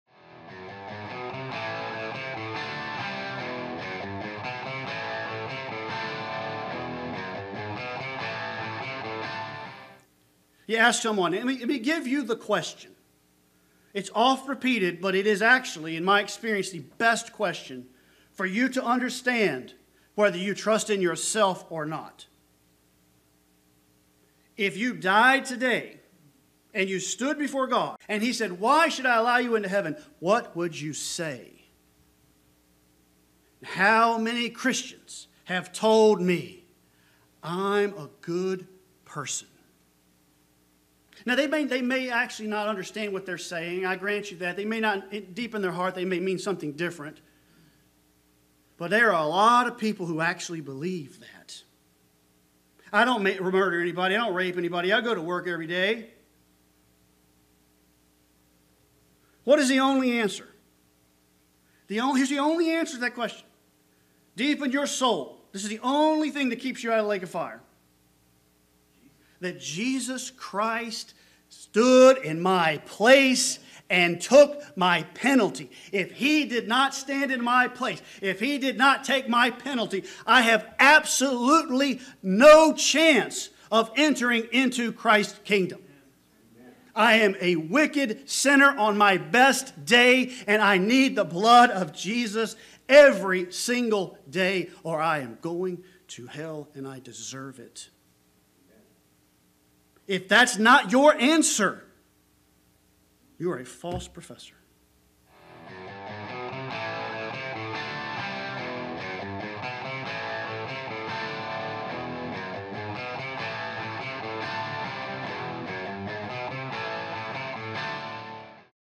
The Best Question to Determine if You are a False Professor
In this clip, Wes Moore explains the best question to ask yourself to determine if you are a true believer or false professor. How would you answer this question?